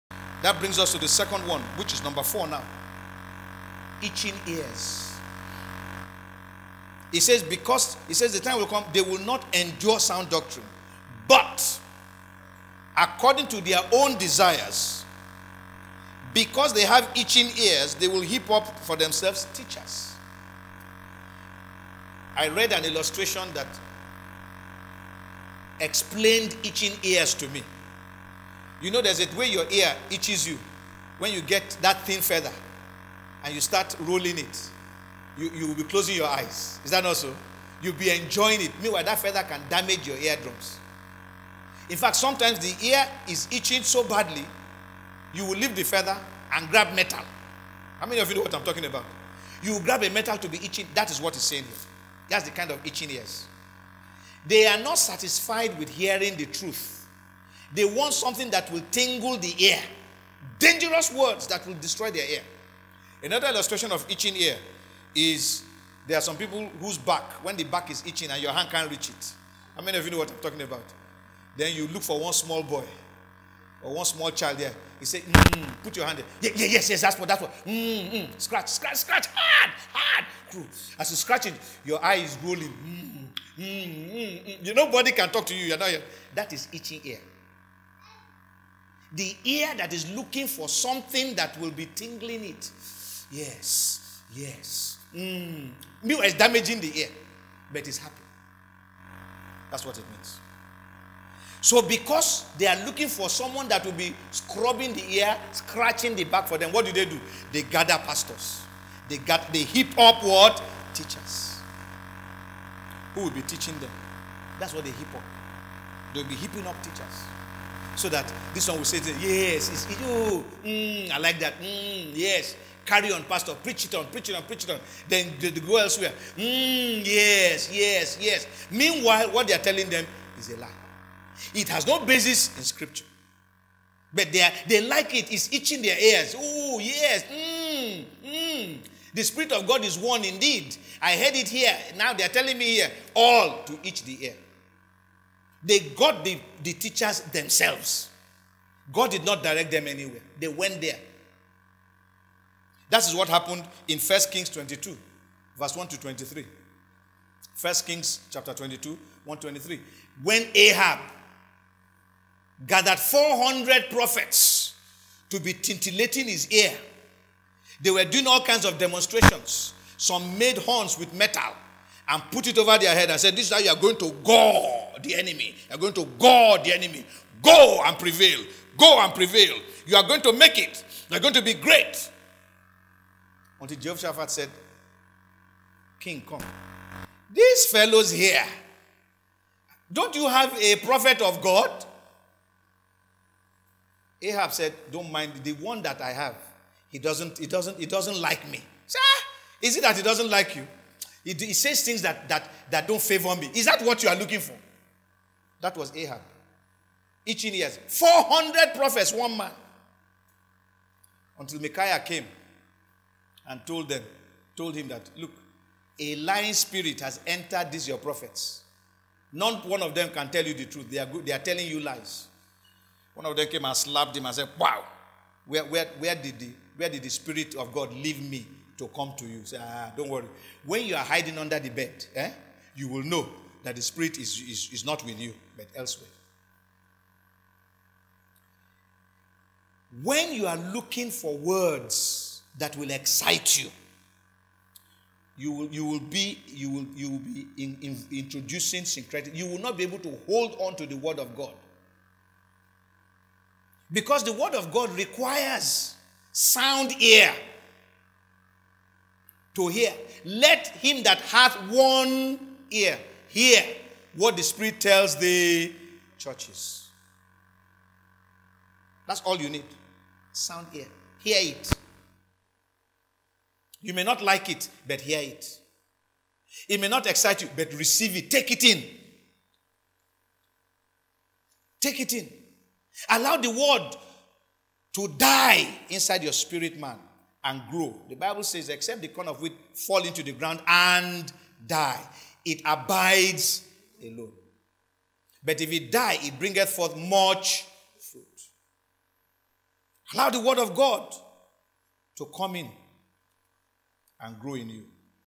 The Challenge of Itching Ears
"Itching ears" is a phrase that speaks of a propensity to want to hear what you want to hear--something that tickles the ear, as against something that you need to hear to straighten you out in the course of your walk with God. When itching ears develop, the hearer decides on what he wants to hear, and therefore, susceptible to being syncretic.